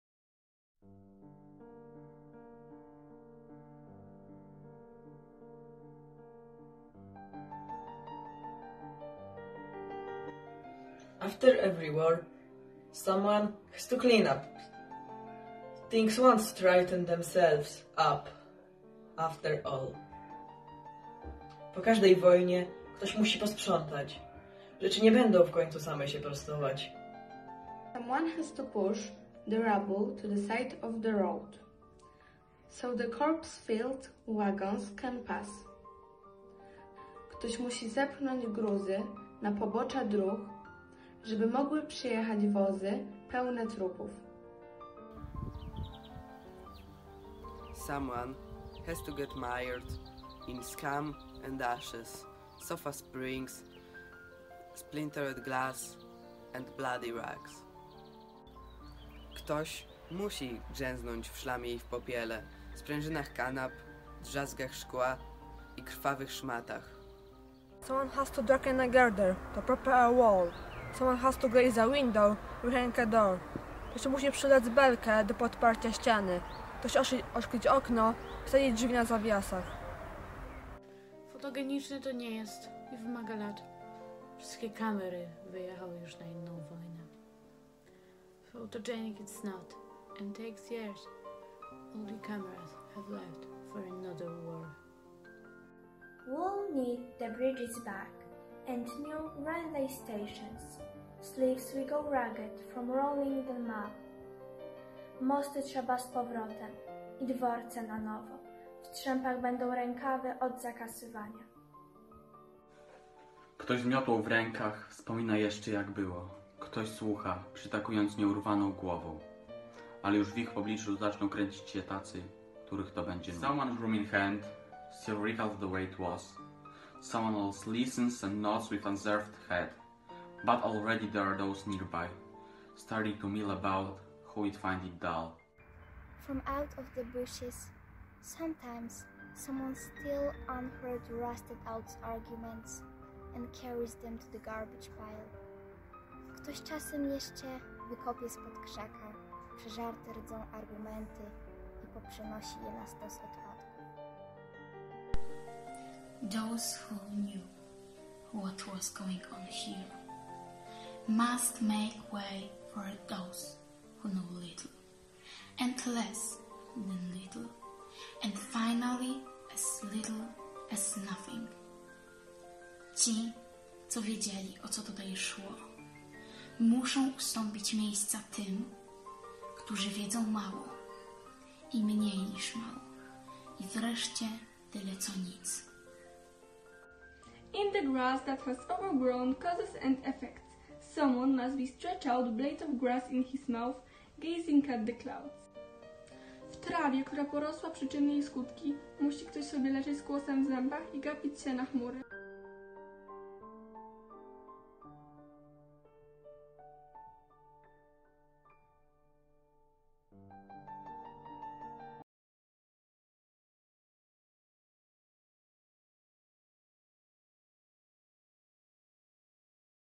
The End and the Beginning by Wisława Szymborska - STO Primary School in Ciechanów
The End and the Beginning by Wisława Szymborska recided by the students of STO Primary School in Ciechanów on the occasion of Irena Sendlerowa ,the school's Patron Day.